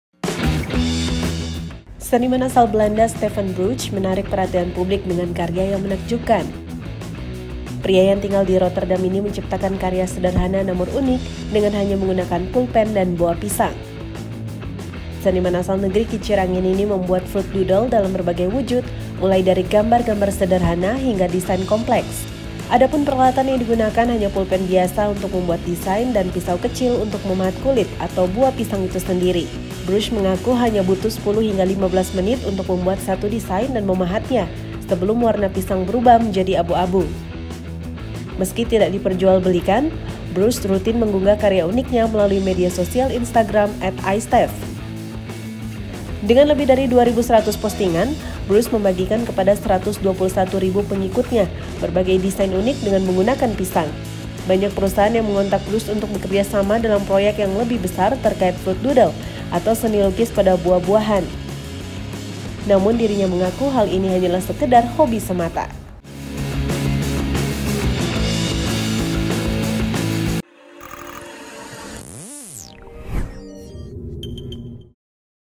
Seniman Belanda Buat Karya Unik dari Buah Pisang
Seniman asal Belanda Stephan Brusche membuat karya unik dari pisang. Dirinya hanya membutuhkan pulpen dan pisang untuk membuat karya unik tersebut.

Download aplikasi VisualTV.Live:
Akses dan Follow Official Account VISUALTV.LIVE di:
- Line: @visualtvlive
- Path: @visualtvlive